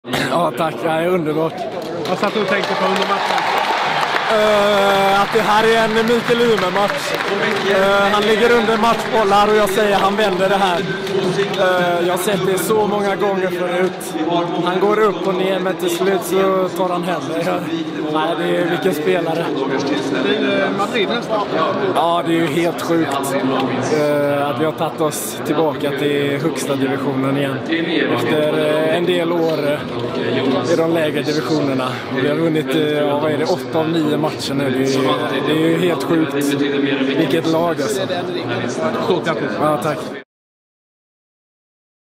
Markus Eriksson lycklig efter segern
Markus Eriksson direkt efter avancemanget till Davis Cup Finals.